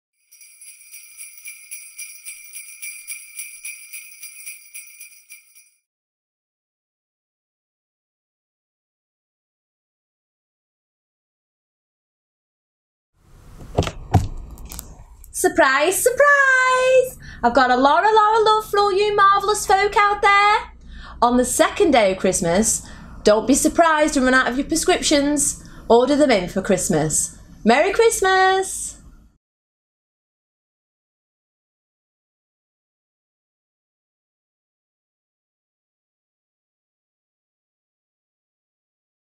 The Pumping Marvellous 12 Days of Christmas - The Second Day
In our second Christmas special, Clare our corporate and grant fundraising manager impersonates a famous celebrity and gives you a special tip for managing your heart failure this Christmas, through the marvellous window. Can you guess who it is? HoHoHOPE you can!
Have a Pumping Marvellous Christmas everyone! Our closed support group for patients and carers is open throughout season.